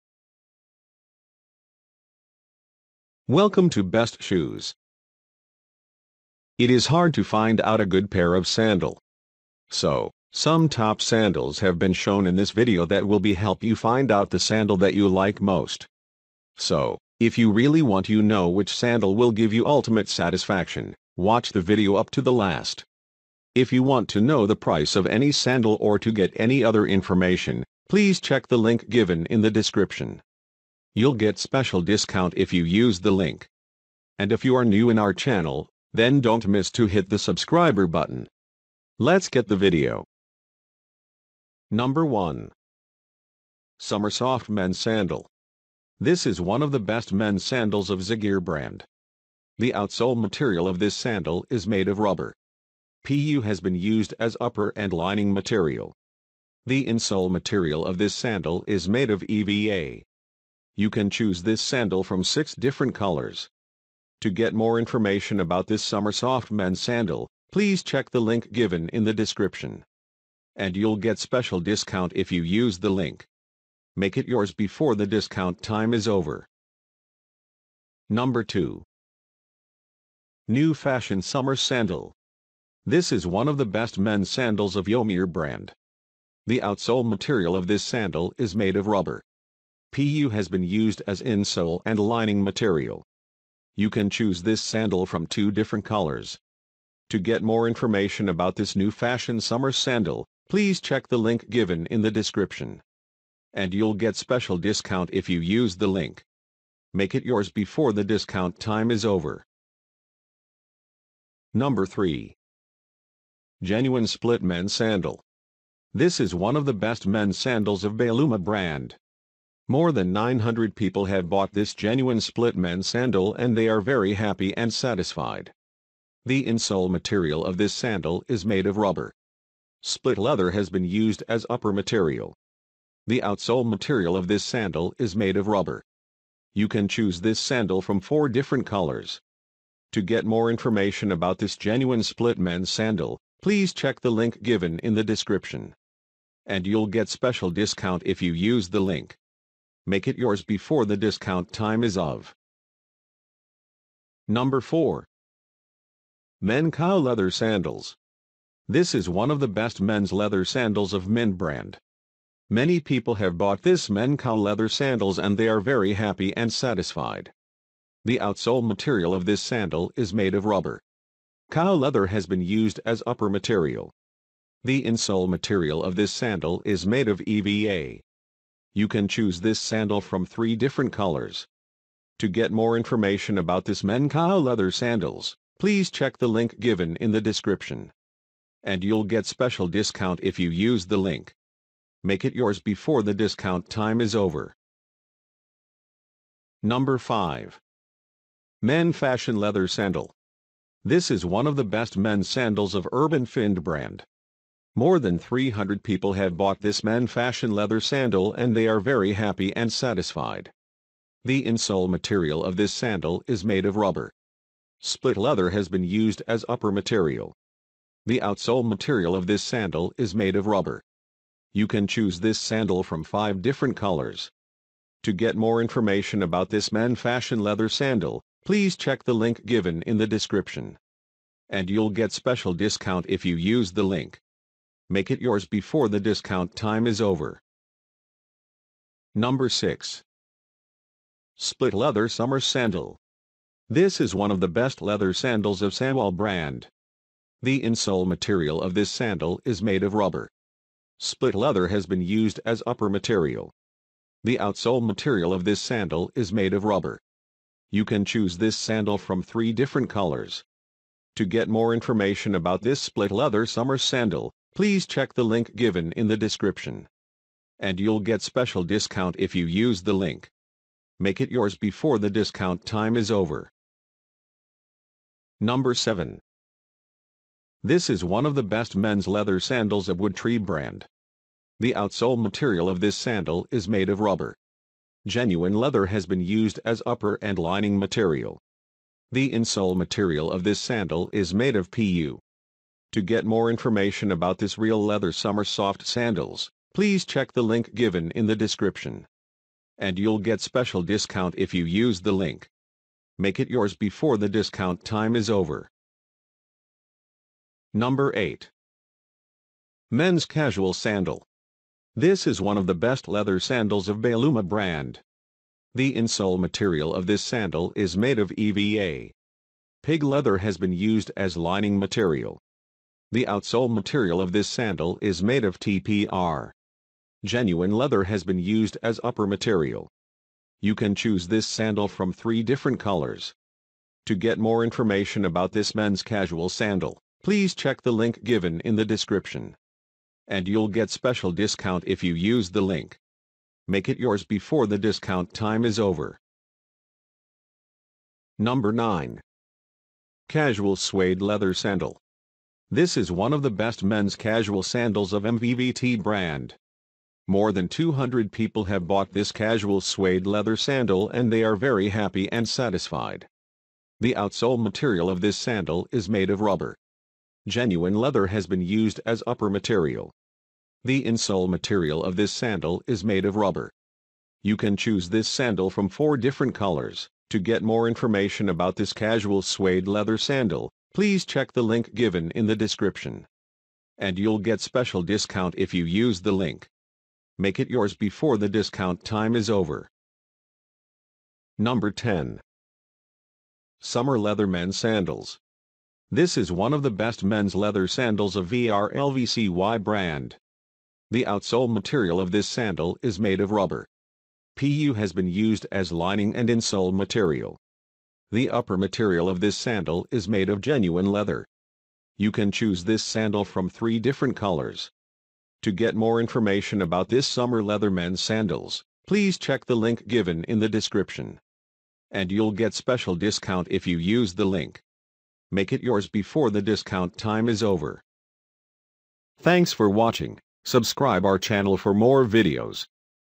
Latest Men’s Sandals Designs 2024
Latest Men’s Sandals Designs For 2024.

This lists all sandals are new and fashionable. You can get any one from this list.

This men’s sandals list can be a good choice if you are really in need of a latest men’s sandals designs and don’t want to invest much money.